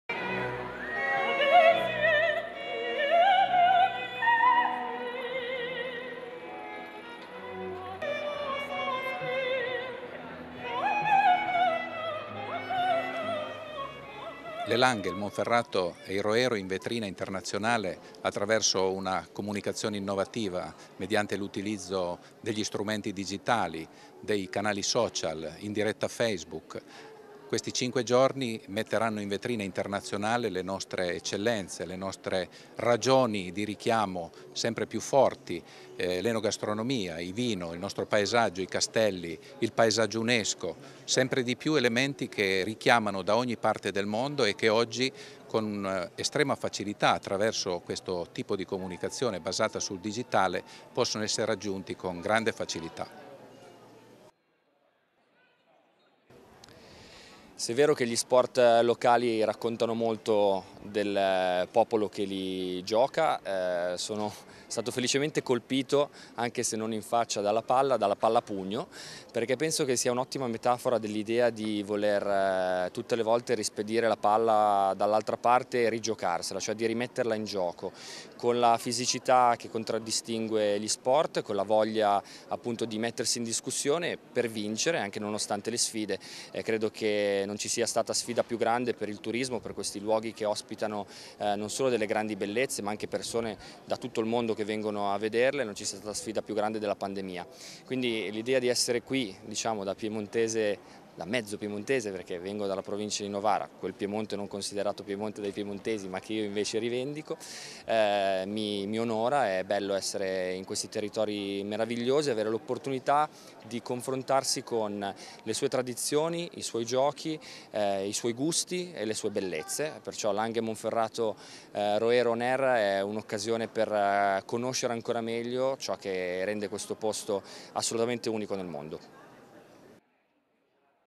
LANGHE, MONFERRATO E ROERO: AL VIA PRIMI EVENTI IN STREAMING
E' partita la kermesse in streaming che permette di far conoscere le eccellenze del territorio delle langhe, del monferrato e del roero. L'iniziativa, andrà avanti tra enogastronomia e sport fino al 21 settembre